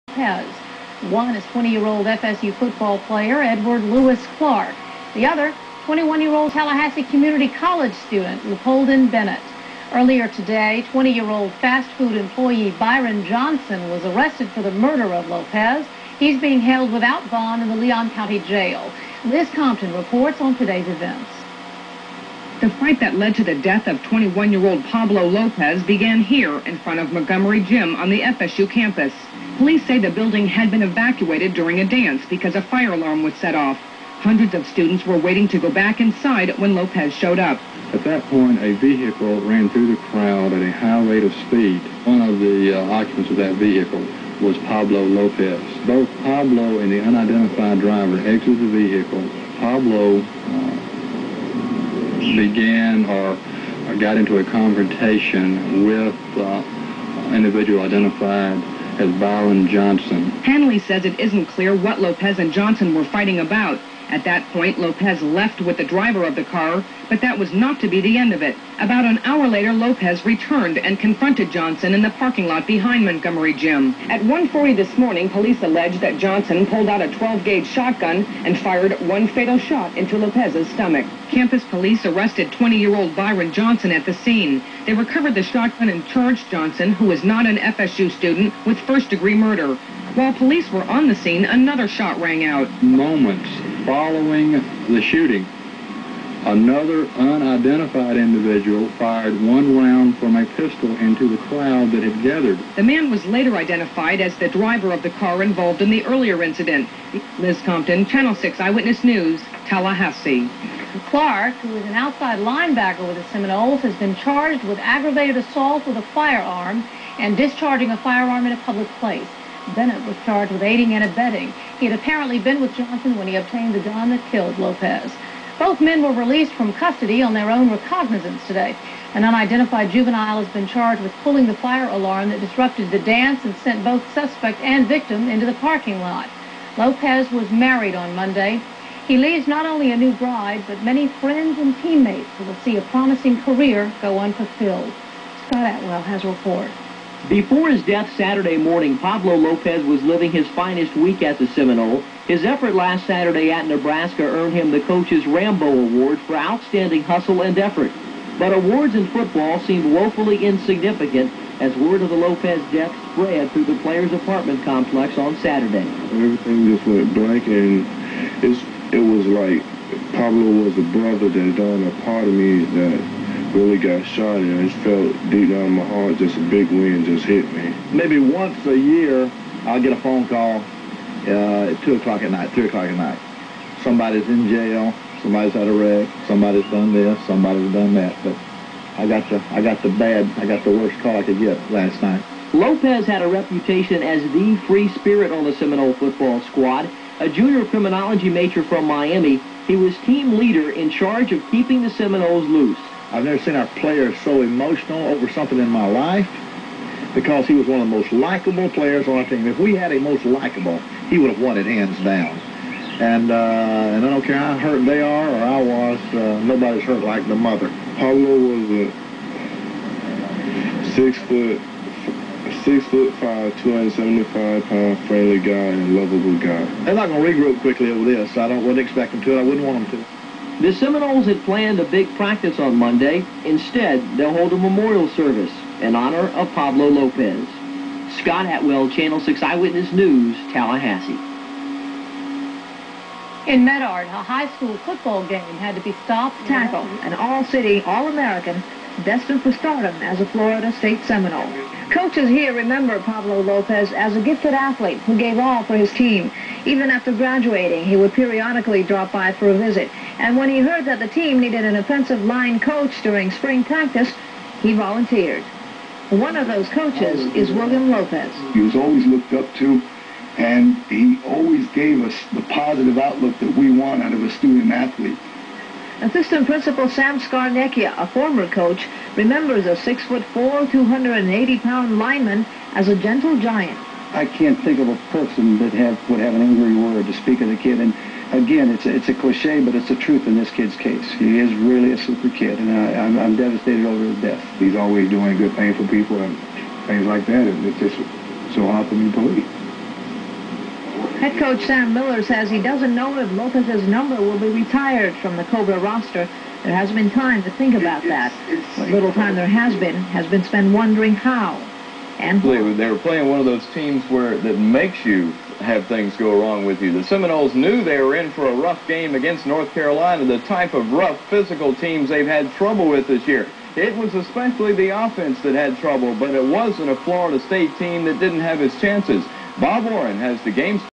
Sad death of Florida State player Pablo Lopez on September 14, 1986
Video of the newscast after the sad death of Florida State player Pablo Lopez on September 14, 1986